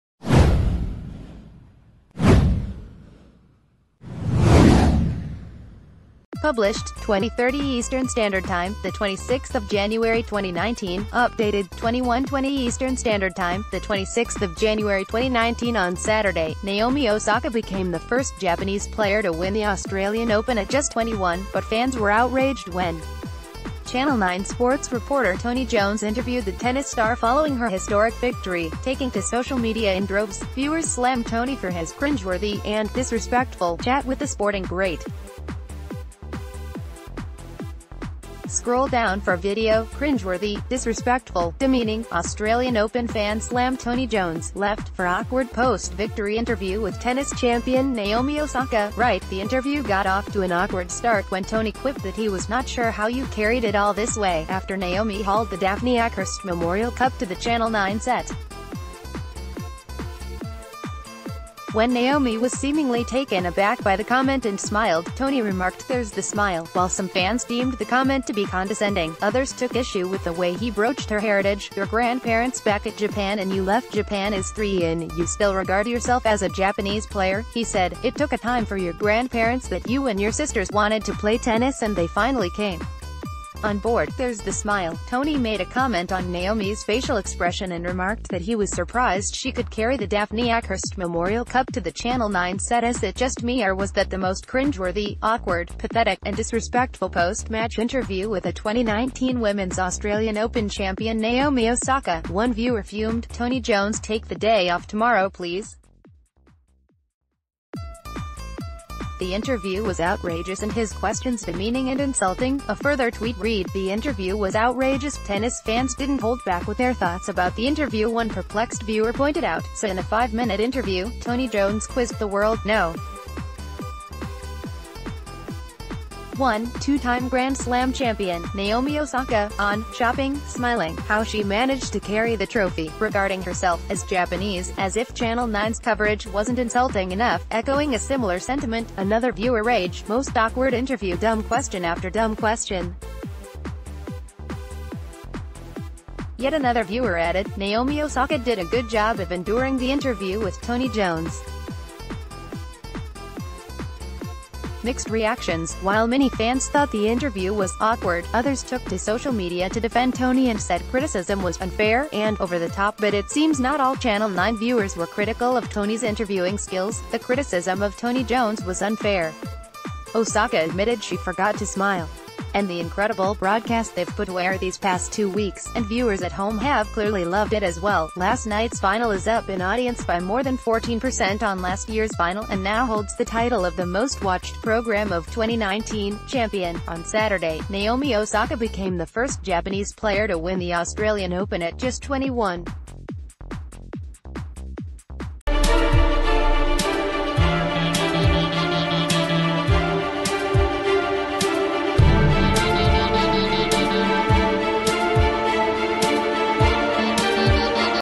Australian Open fans SLAM Tony Jones for interview with Naomi Osaka
Published: 20:30 EST, 26 January 2019 | Updated: 21:20 EST, 26 January 2019 On Saturday, Naomi Osaka became the first Japanese player to win the Australian Open at just 21. But fans were outraged when Channel Nine sports reporter Tony Jones interviewed the tennis star following her historic victory. Taking to social media in droves, viewers slammed Tony for his  'cringeworthy' and 'disrespectful' chat with the sporting great. Scroll down for video  'Cringeworthy, disrespectful, demeaning': Austr...